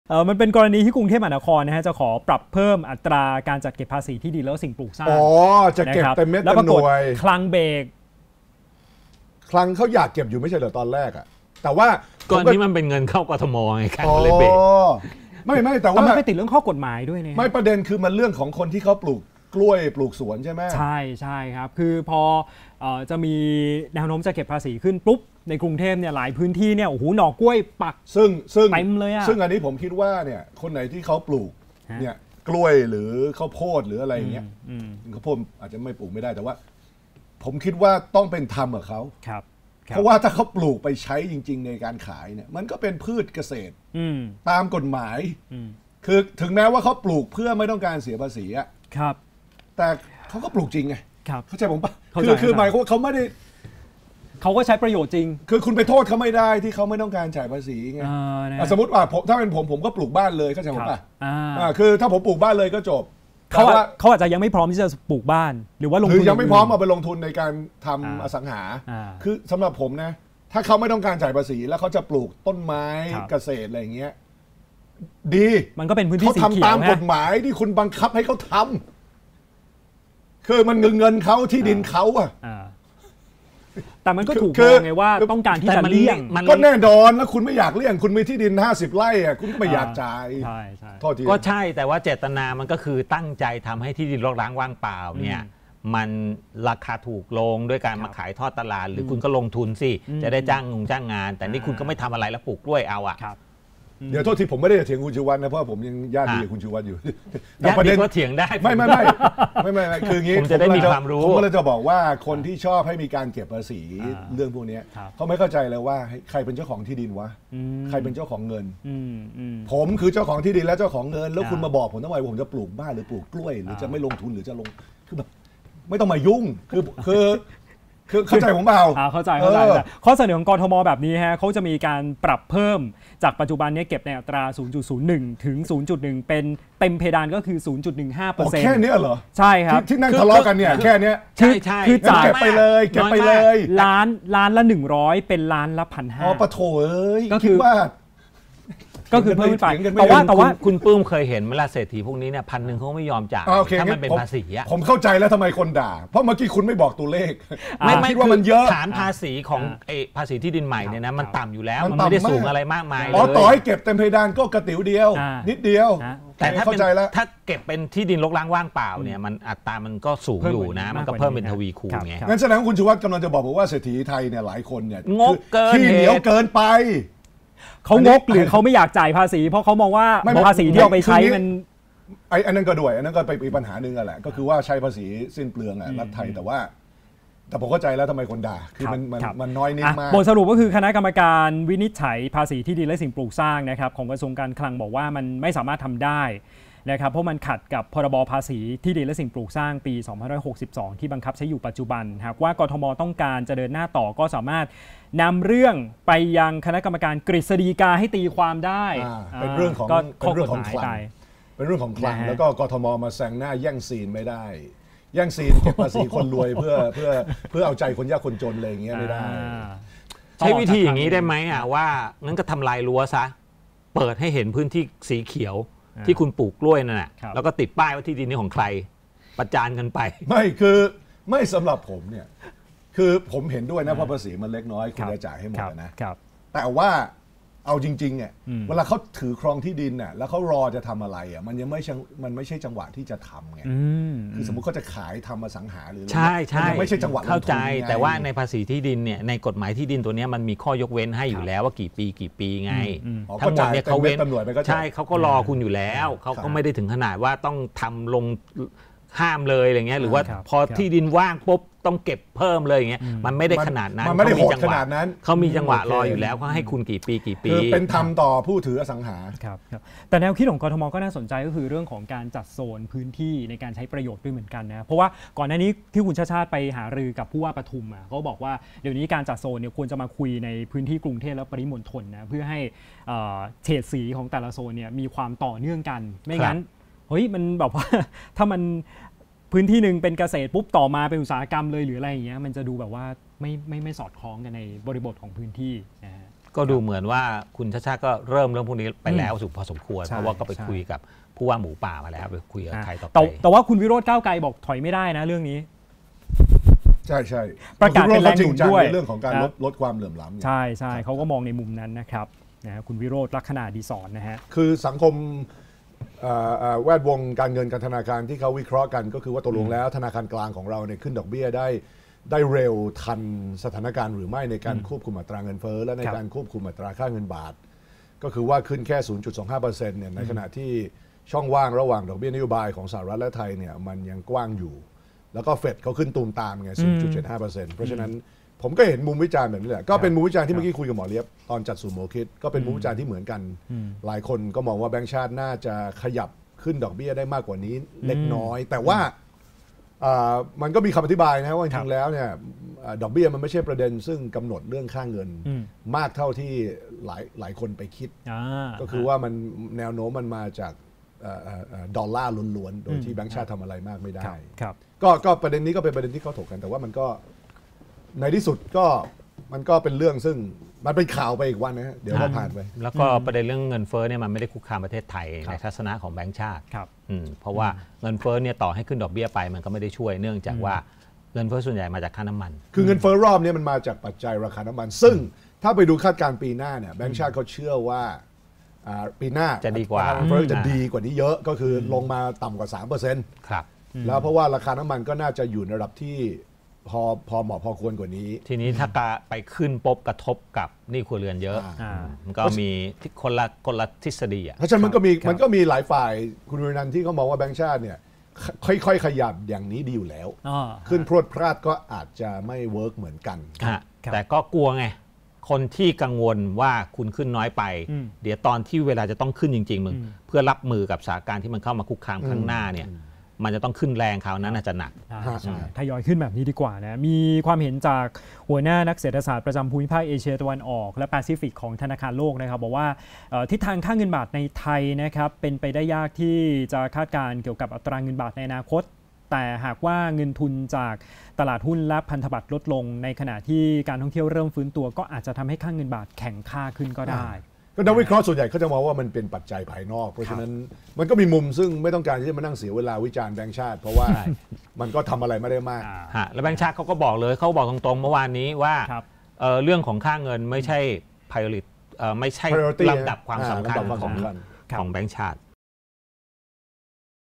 แลนด์ลอร์ดหวานหมู! คลังขวาง กทม. เพิ่มภาษีที่ดินเปล่าปลูกสวนกล้วย
หลัง กทม. นำโดย 'ชัชชาติ' ปิ๊งไอเดีย เพิ่มภาษีที่ดินและสิ่งปลูกสร้าง ประเภทที่ดินเกษตรกรรมแยกย่อยตามโซนผังเมืองรวม จากปัจจุบันเก็บในอัตรา 0.01-0.1% เป็นเก็บเต็มเพดาน 0.15% หลังพบ 'เจ้าที่ดิน' นำที่ดินเปล่าไปปลูกกล้วยกันเต็มเมืองหวังเลี่ยงภาษี แต่เมื่อเรื่องนี้ถึงหู 'คลัง' กลับสั่งเบรก อ้างขัดกฏหมาย งานนี้เจ้าที่ดินไทยโล่งอก ยังไม่ต้องจ่ายภาษี ทั้งที่ถ้าต้องจ่ายก็มีกำลังจ่ายได้ แต่ทำไมไม่อยากจ่าย